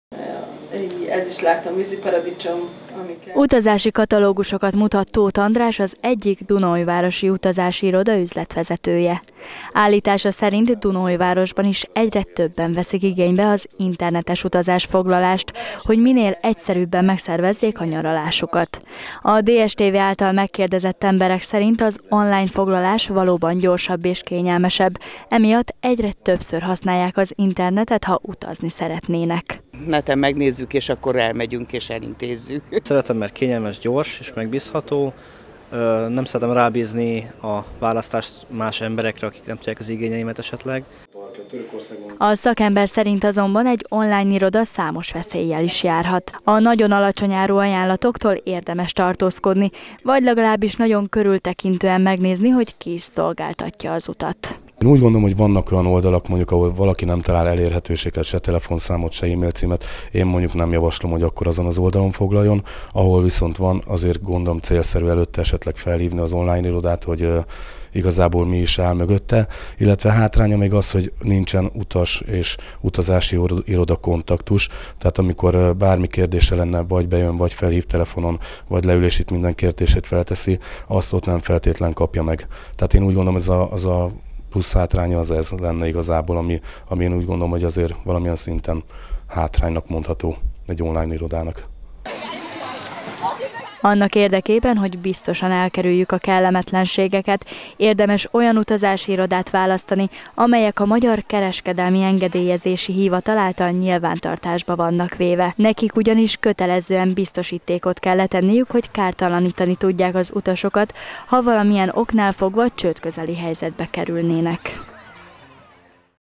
Útra fel!
Dunaújvárosban is egyre népszerűbb az online utazásfoglalás.  Az emberek szerint sokkal gyorsabb, kényelmesebb és egyszerűbb.  Azonban számos veszélyt is rejthet magában.